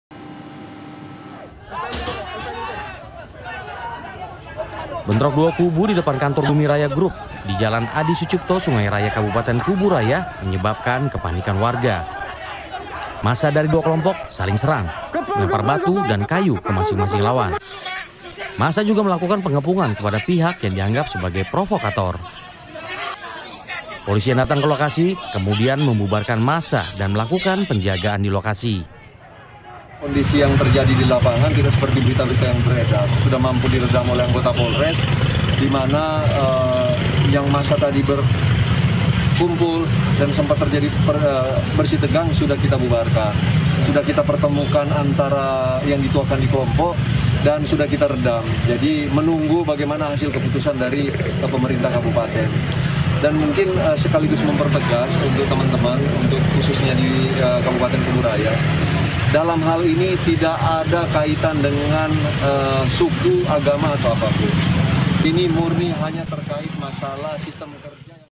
Bentrokan Buruh Bongkar Muat, Berebut Lahan Kerja
Dua kelompok buruh bongkar muat di Kabupaten Kubu Raya, Kalimantan Barat, terlibat bentrok. Mereka saling melempar batu dan kayu, lantaran merebutkan lahan kerjaan.